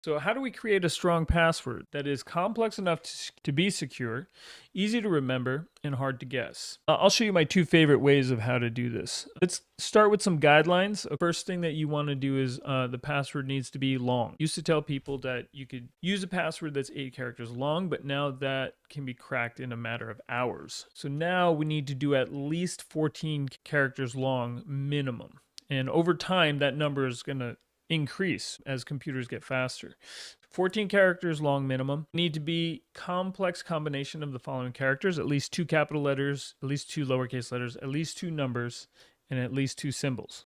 How to Create a Secure and Memorable Password: 2 Effective Strategies
Creating a strong password that is both secure and easy to remember can be a challenge. In this video, I share my two favorite ways to create complex and strong passwords. Follow these guidelines to ensure your online accounts are well-protected.